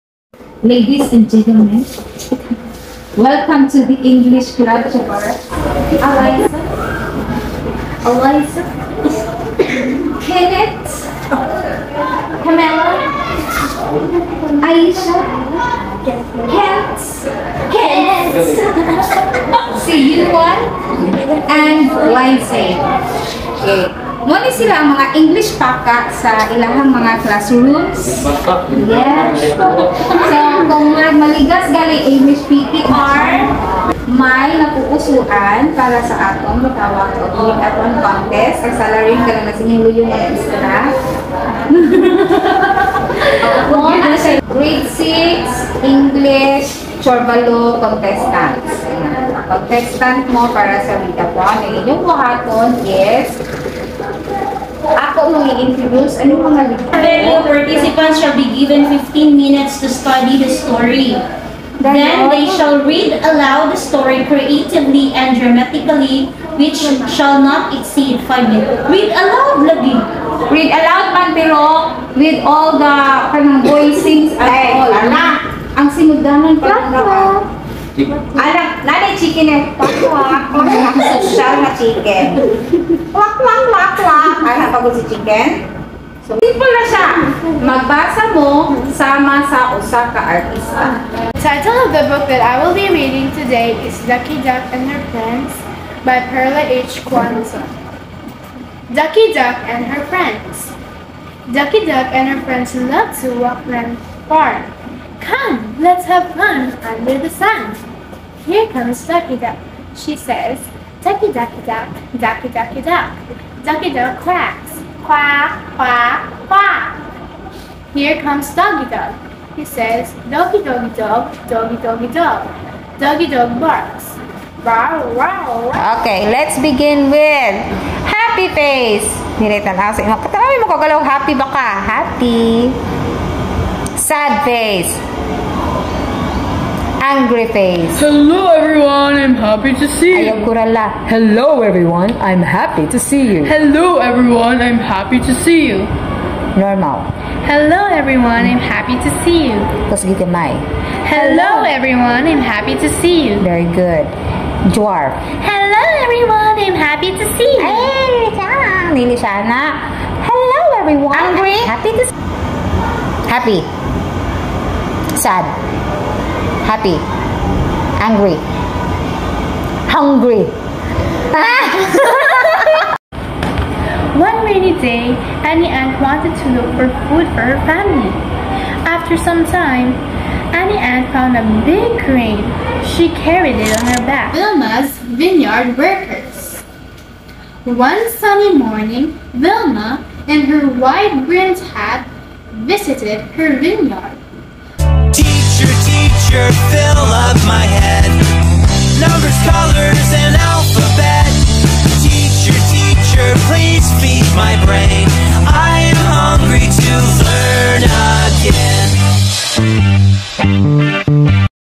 Read-a-thon English 6 | Oral Reading Interpretation
One of the events in the Festival of Talents this year is Read-a-thon for Grade 3, 6, and 10. Representing Grade 6, here's my entry of coaching for the first time the Oral Reading Interpretation. We reached the Division Level, third place, and the experience was more than that.